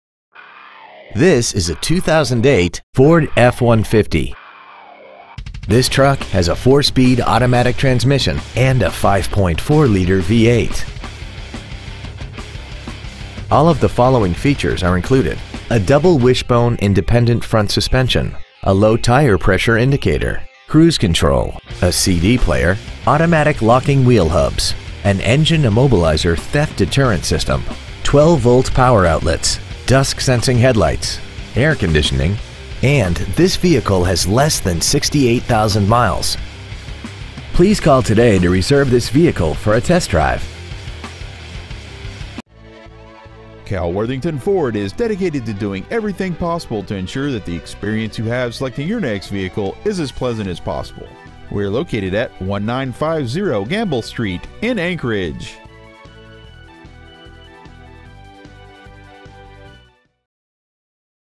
2008 FORD F-150 Anchorage AK
Year : 2008
 Make : FORD
 Model : F-150
 Engine : 5.4L V8 24V MPFI SOHC FLEXIBLE FUEL
 Trans . : 4-SPEED AUTOMATIC
 Exterior : WHITE
 Miles : 67,513

 Stock : Q0623